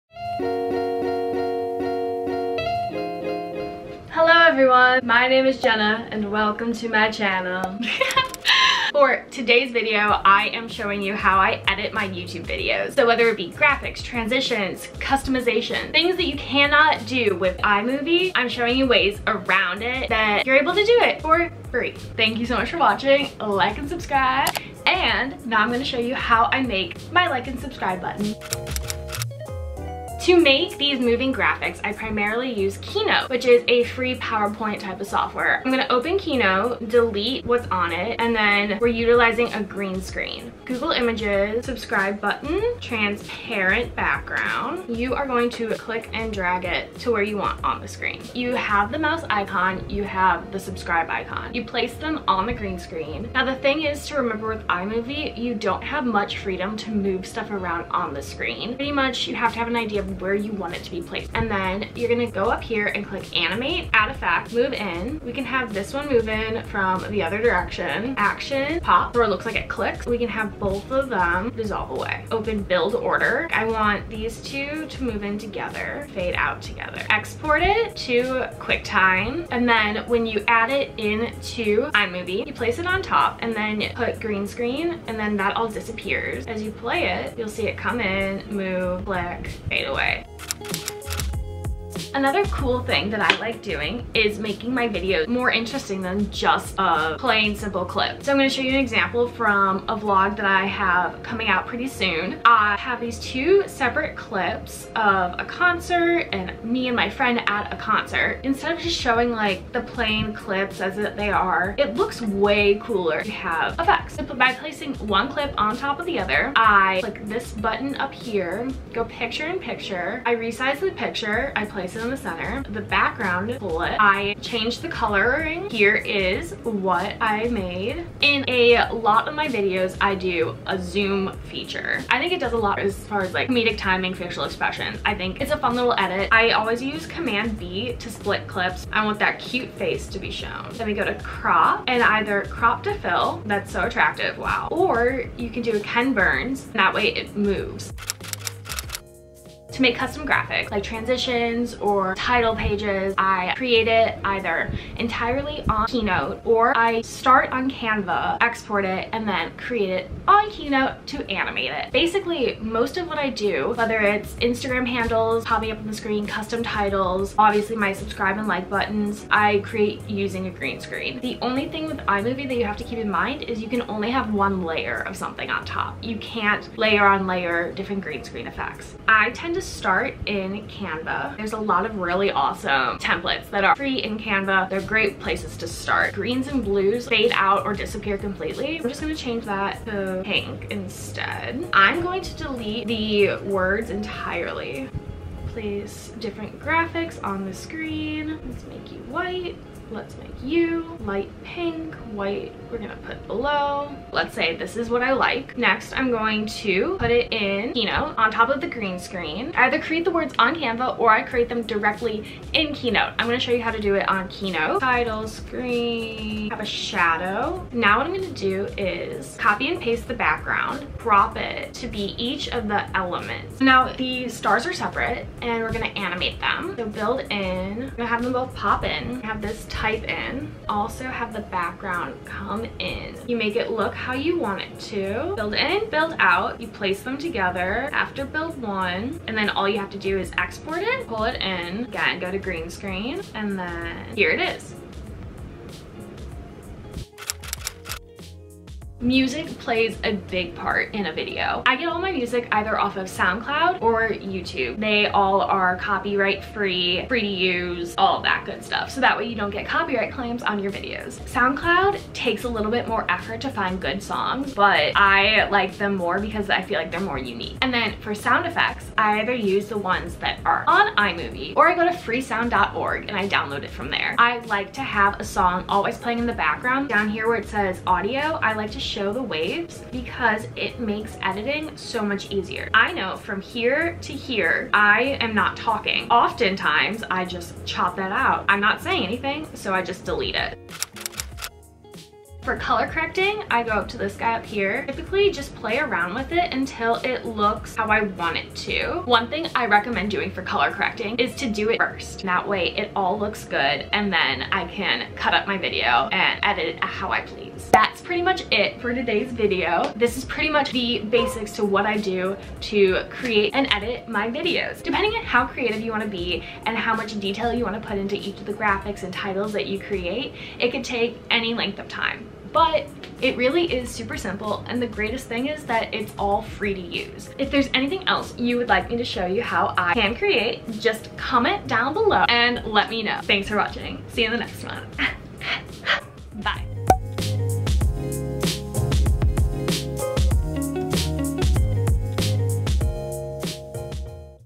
iMovie Editing HACKS- Custom titles , Professional editing & MORE
HI FRIENDZZZ! In today's video, I am going over some of my iMovie 2019 editing hacks for how I edit my YouTube Videos! Custom Title cards, green screen graphics, canva, keynote, soundcloud, sound effects AND MORE! LIKE SUB FOLLOW LOVE YOU

iMovie
Keynote
SoundCloud.com